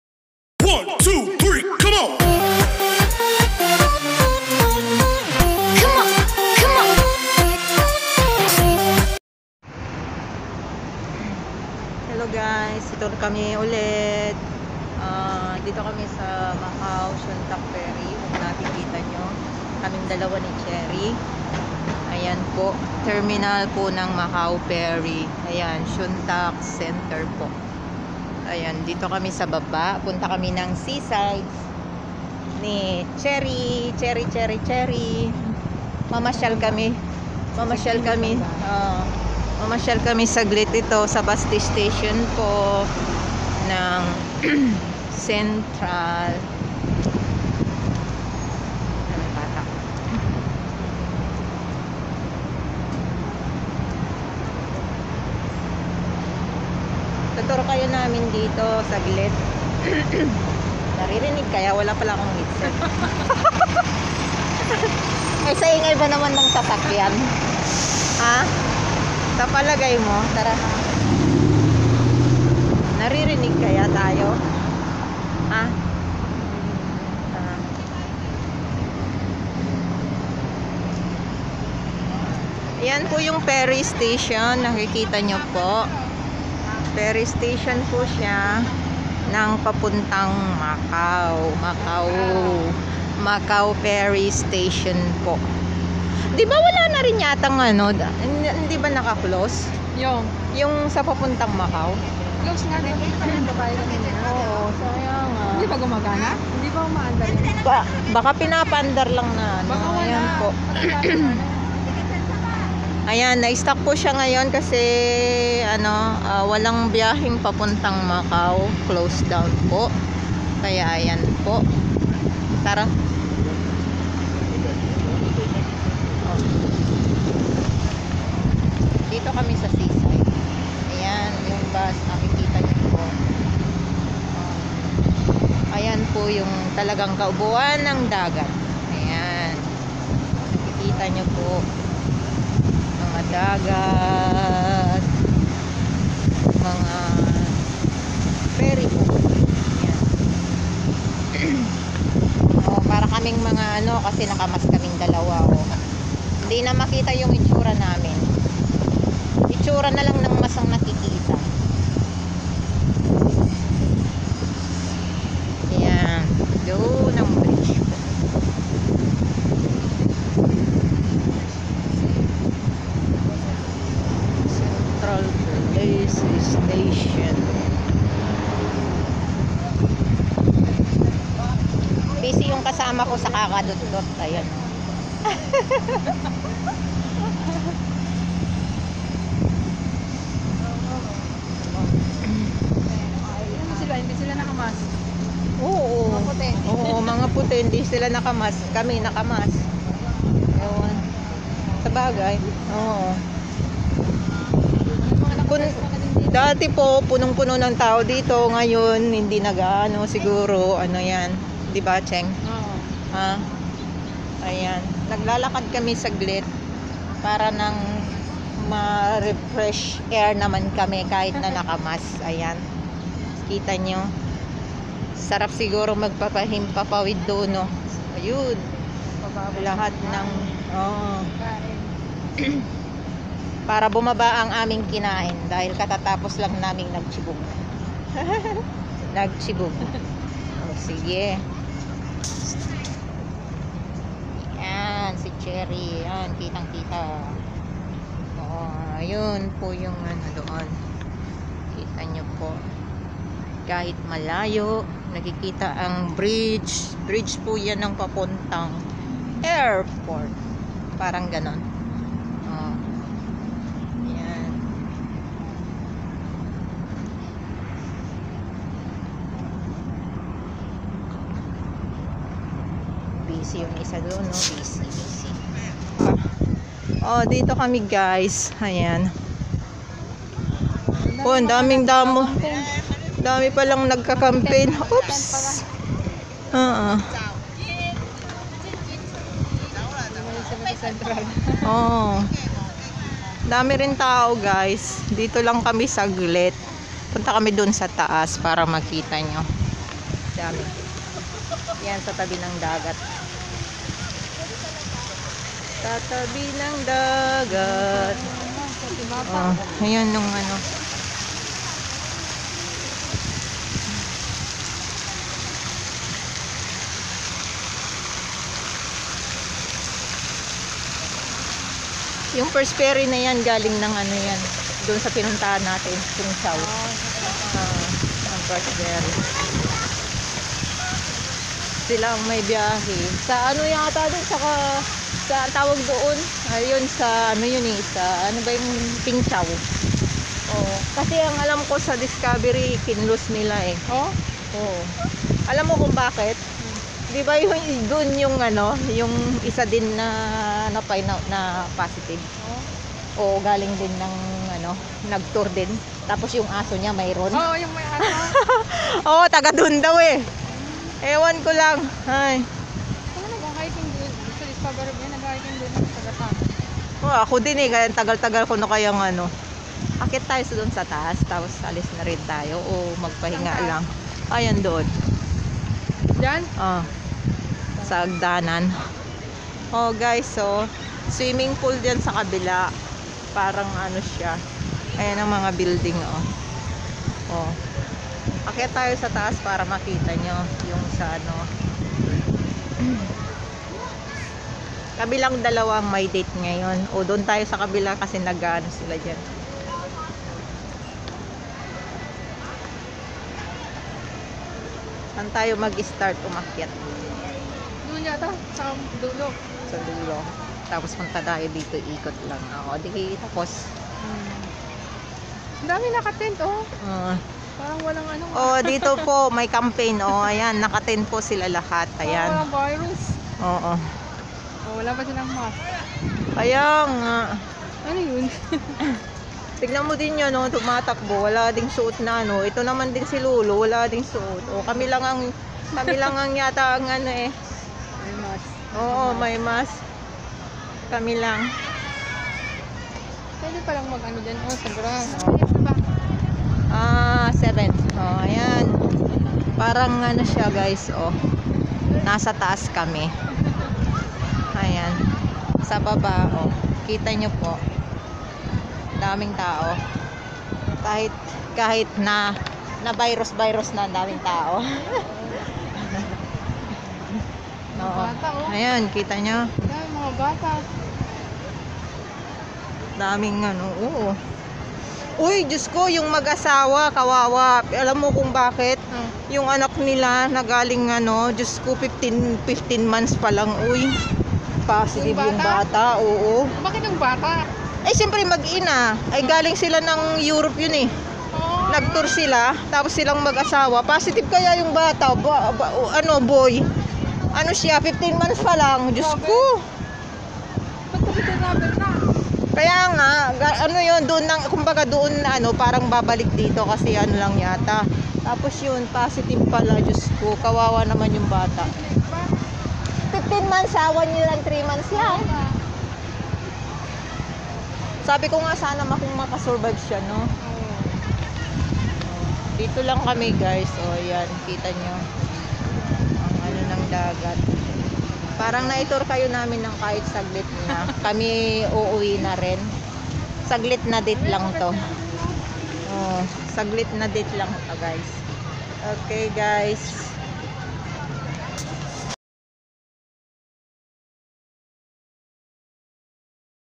ShuengWanMacauFerryStationBonding&ShowingArounAtSeaSideWalkAndPark
ShuengWanMacauFerryStationBonding&showingAroundAtTheSeaSideWalkAndPark&JocelyVlogs
Is About  One Place In Hongkong Called Shueng Wan  Macau Ferry Station.